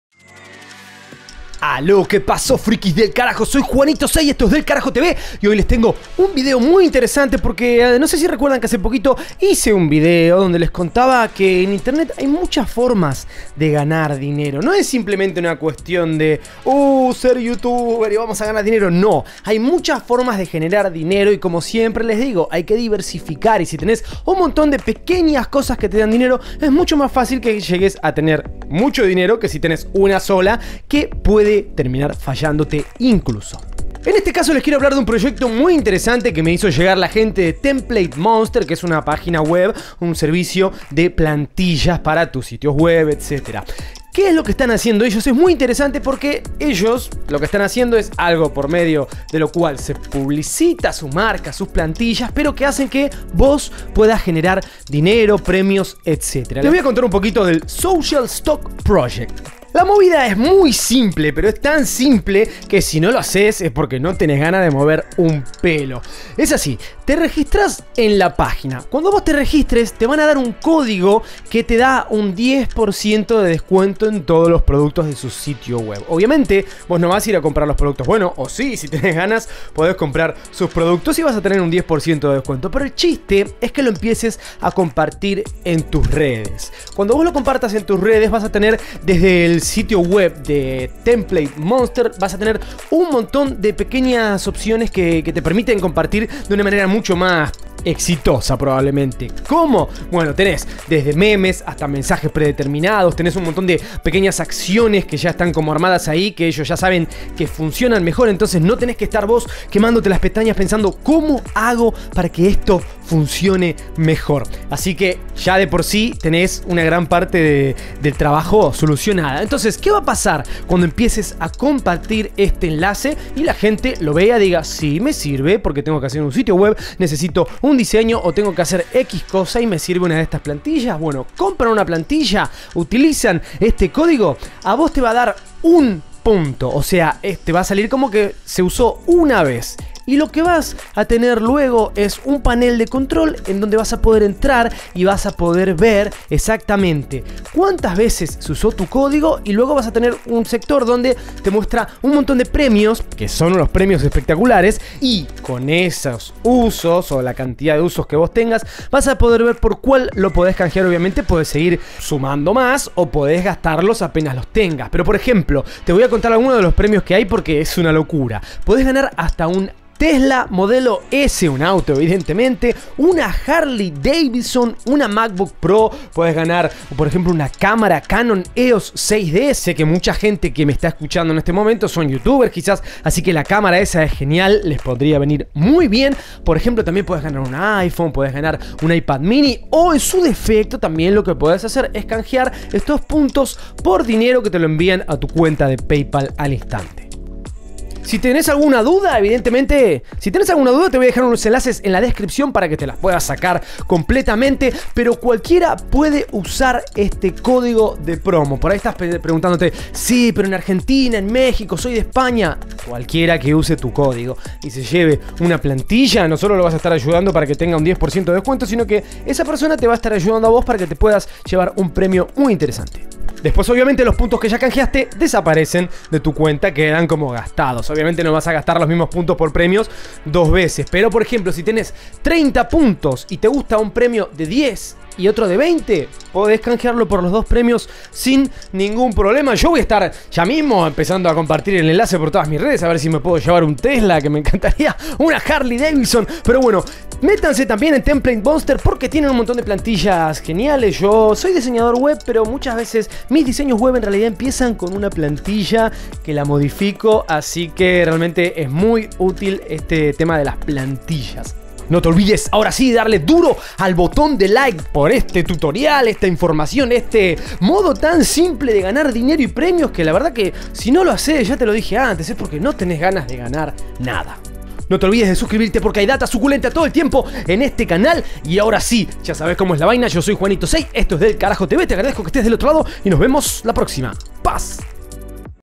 GANA DINERO Y PREMIOS EPICOS CON TUS REDES SOCIALES!!! | Social Stock Project de TemplateMonster
Como siempre, tratando de hacer un bien por los Frikis Delcarajo ayudando a que puedan generar dinero o, si trabajan duro, hasta tener un auto... ¡Tremendo!

* Hey, Friki Delcarajo... ¿Que haces tan abajo? El boton de like esta mas arriba ( ͡° ͜ʖ ͡°)